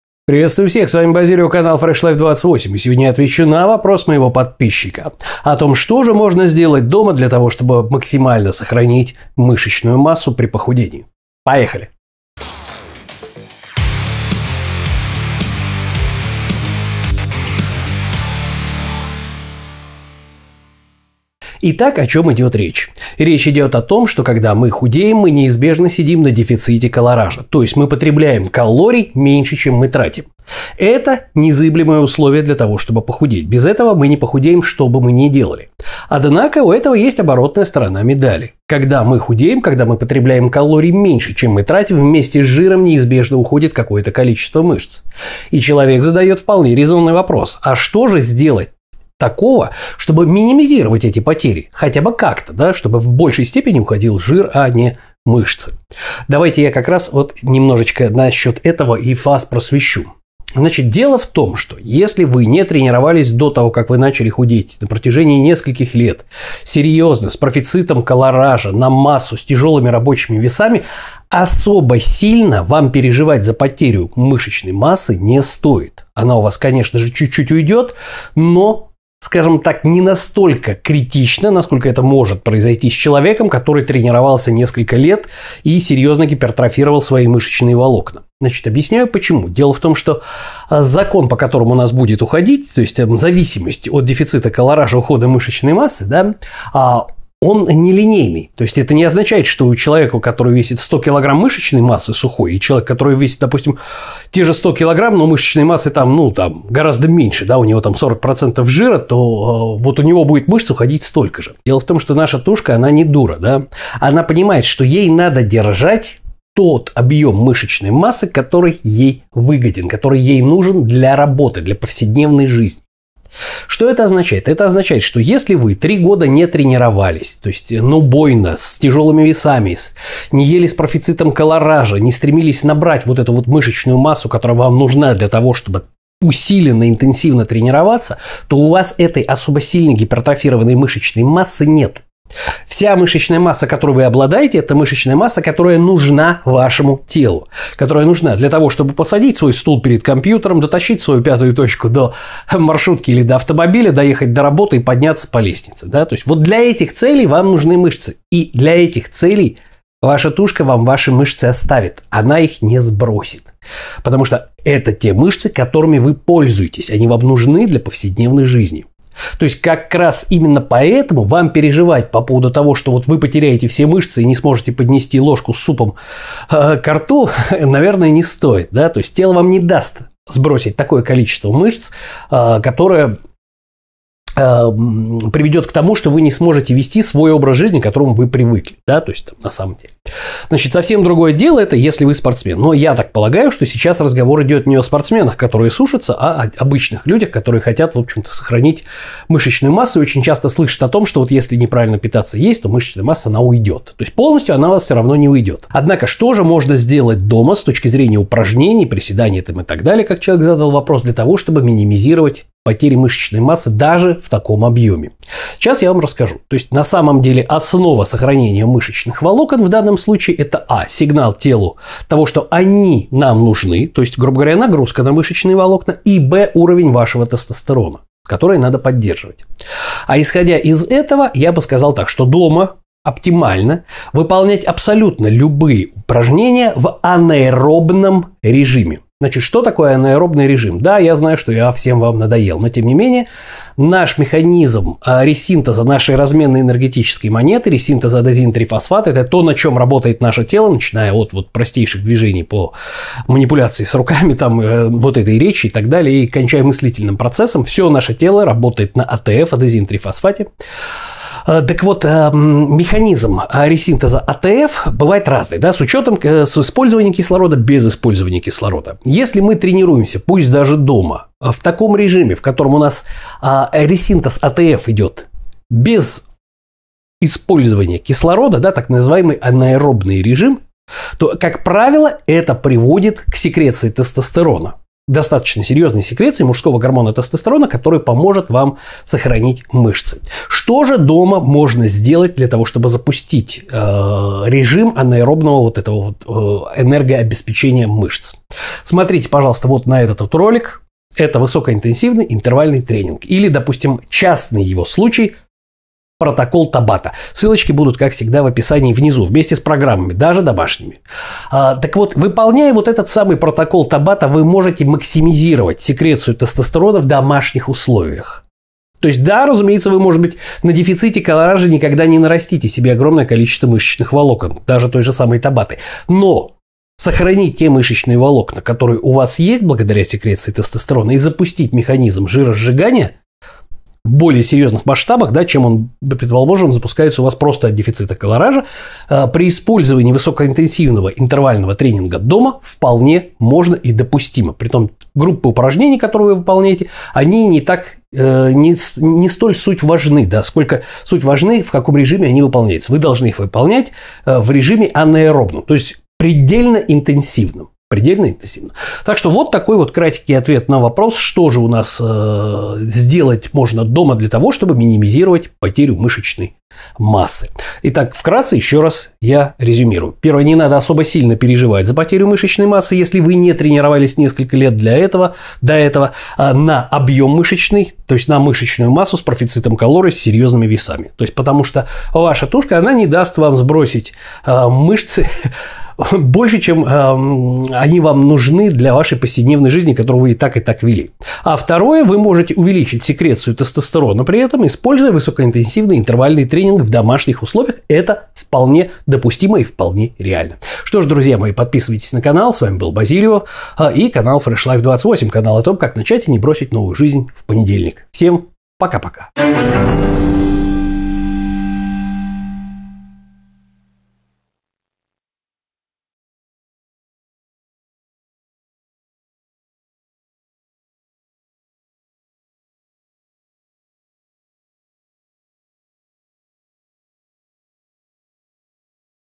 как тренироваться дома. что нужно для сохранения мышц
как тренироваться дома.что нужно для сохранения мышц. Речь идет не о том, как накачаться дома - я в это слабо верю. Как правильно тренироваться в домашних условиях, что бы не сжечь мышцы во время похудения. Какие домашние упражнения эффективны и как их выполнять,что бы не сжечь мышцы, пока вы худеете. Кому надо об этом беспокоиться ?